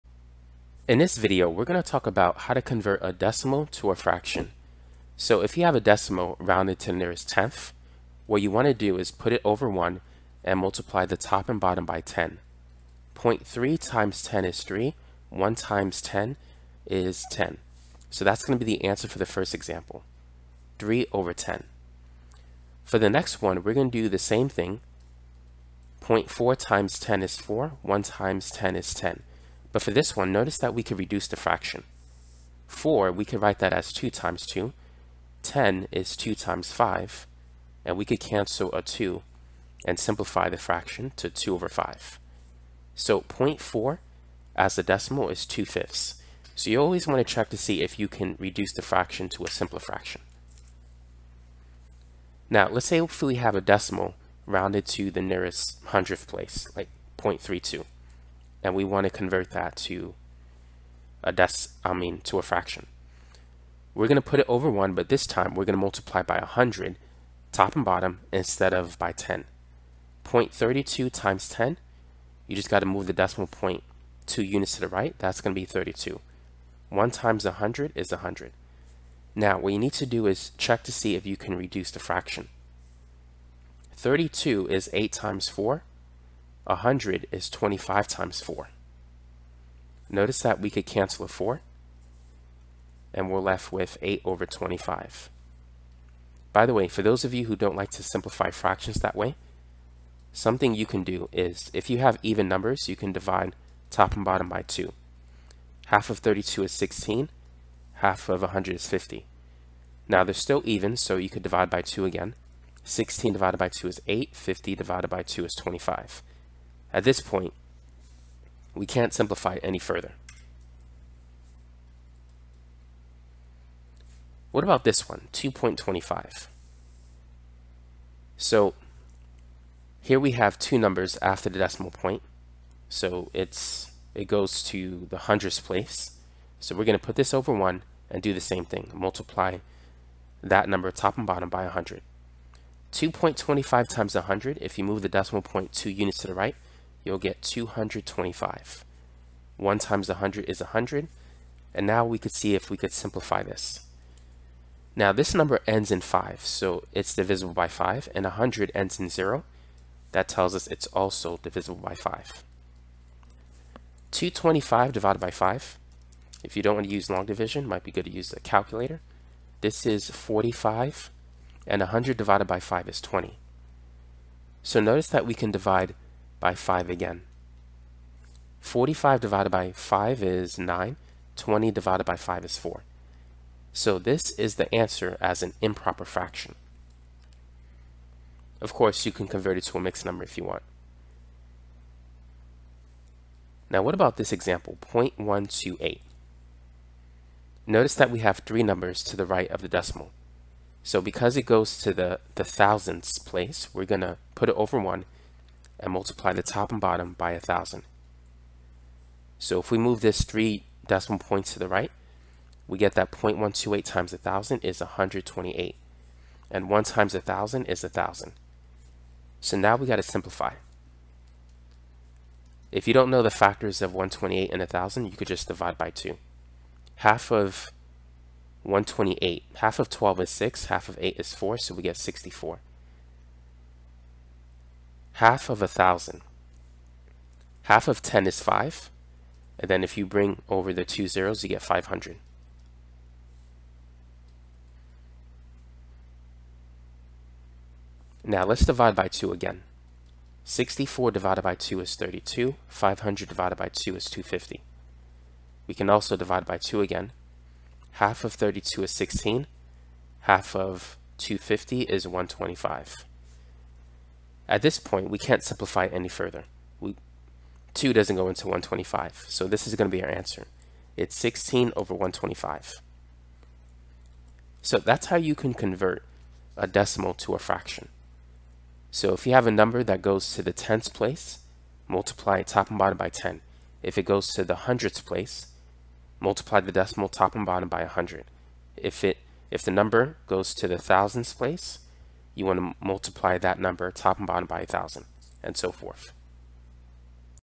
Converting Decimals to Fractions - Math
This math video explains how to convert decimals to fractions.